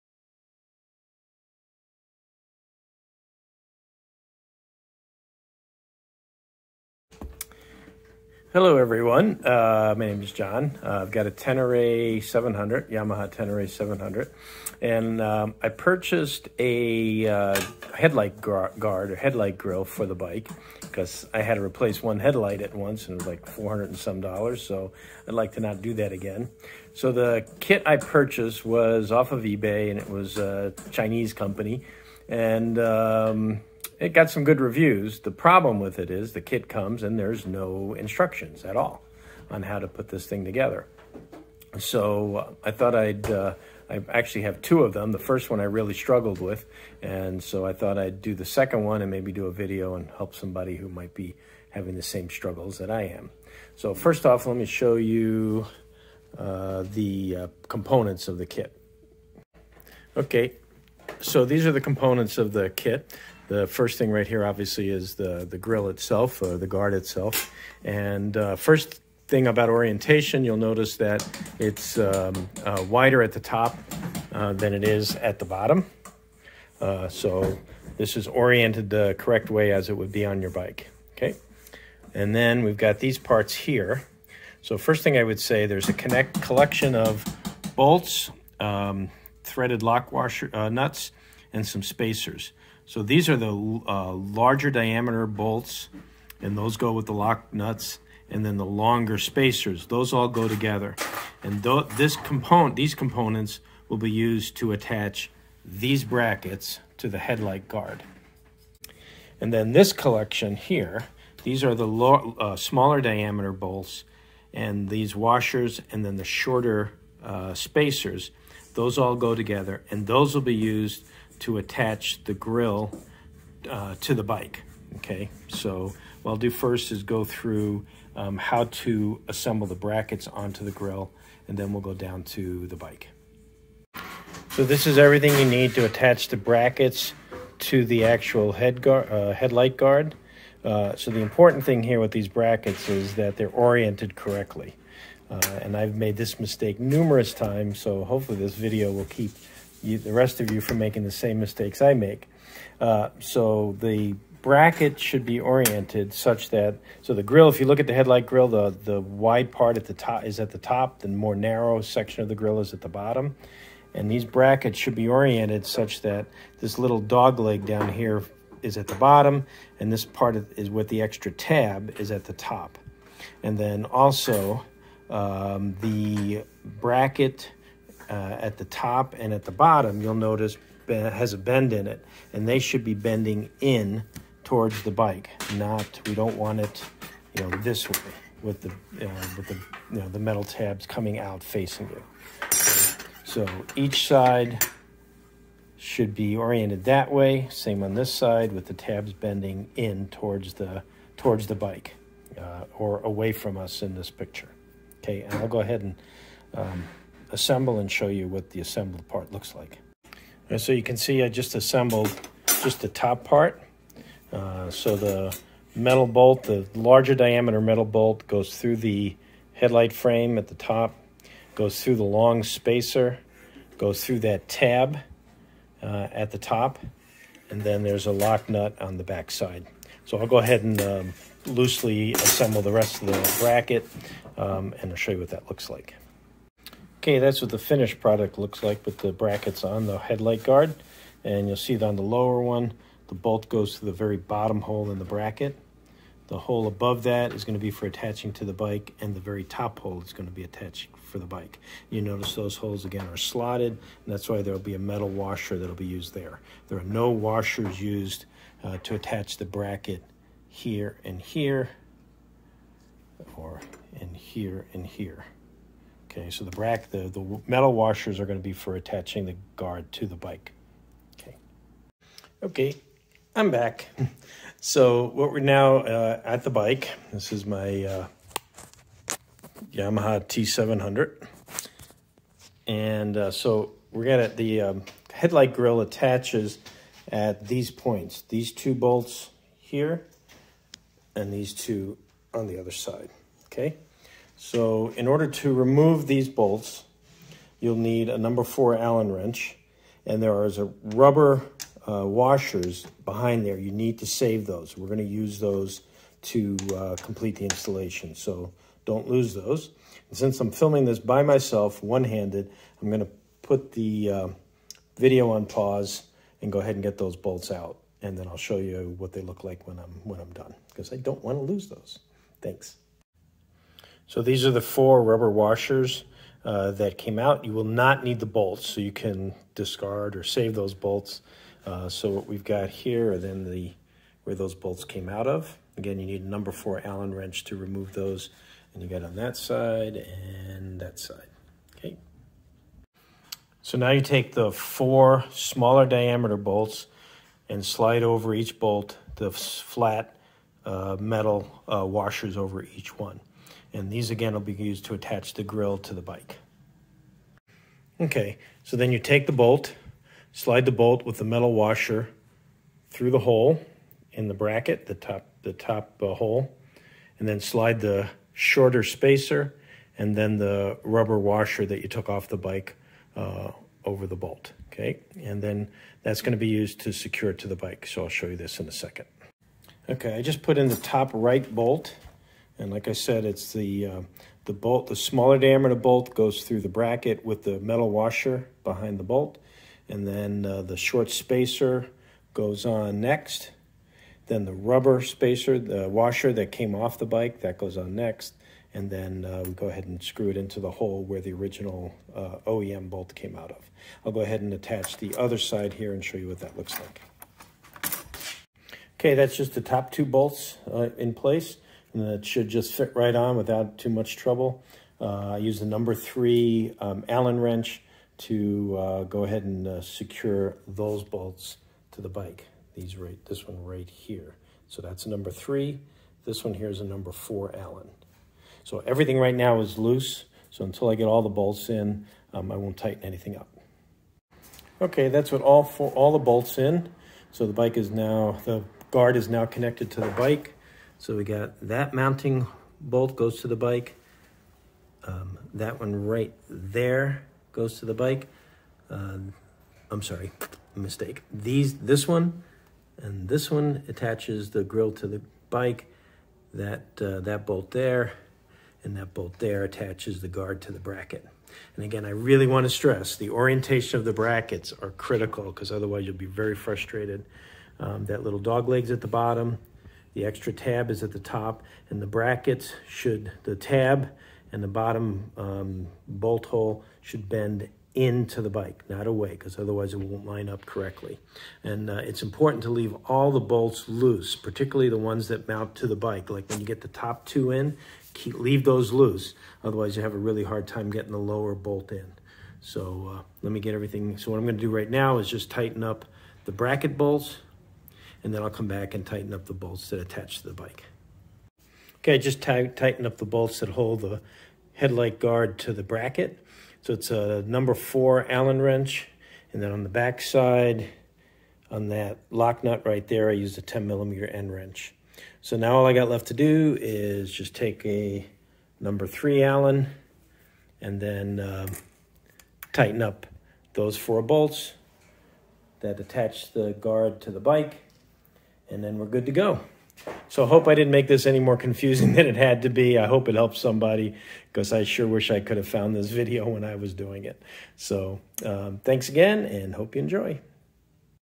Tenere 700 Headlight Guard/Grill Install
Installation of headlight guard/grill protector for 2021 Yamaha T7 Tenere'
The guard kit does not come with any instructions (purchased on ebay) and can be frustrating (at least for me) to install. Hope this helps you not go through the same thing.
Link to item